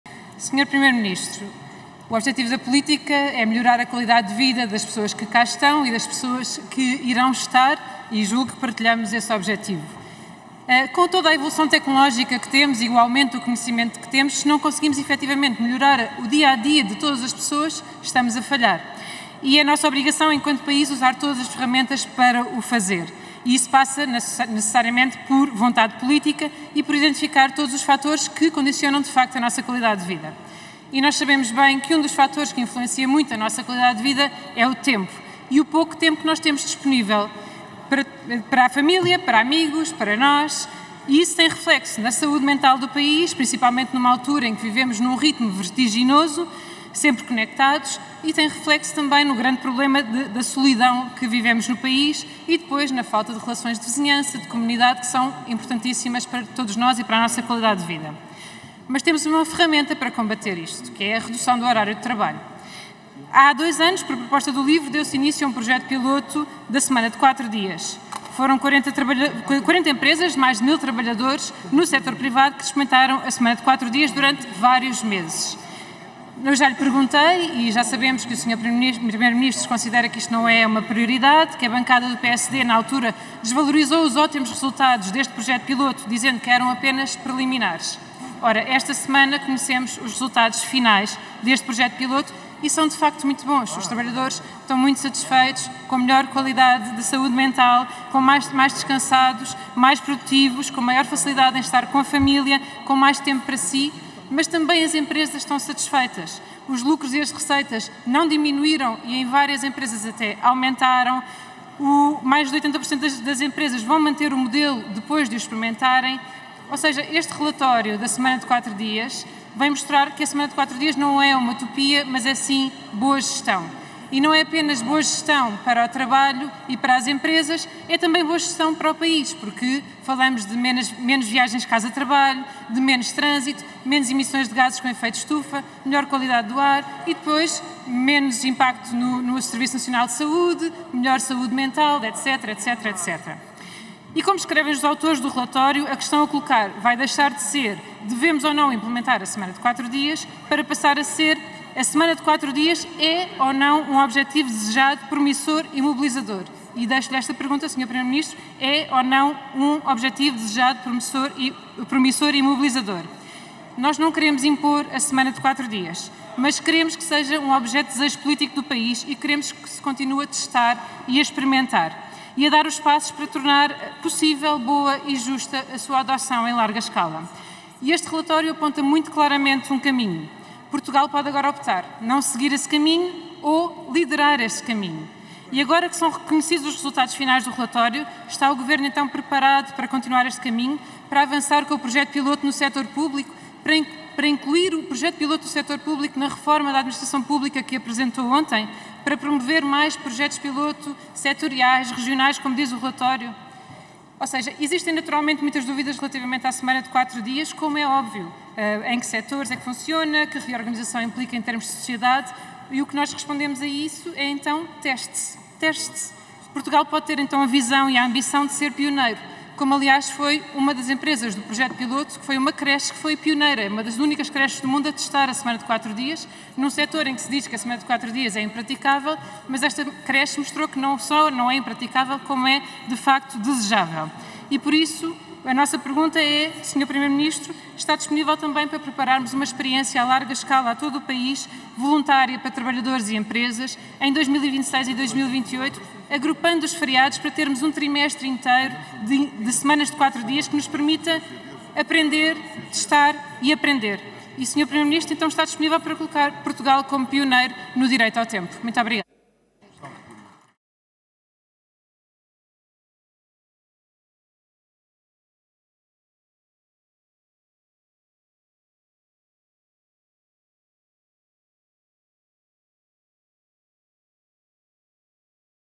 Semana de 4 dias: não é utopia, é futuro
“A vida é mais tempo alegre do que triste. Melhor é ser”, escreveu Adélia Prado, vencedora do Prémio Camões. Perguntámos hoje ao Governo se vai avançar com um projeto-piloto da semana de 4 dias no setor público. Não é utopia, é futuro.

Intervenção da Deputada do LIVRE à Assembleia da República, Isabel Mendes Lopes, na reunião plenária de 26 de junho de 2024.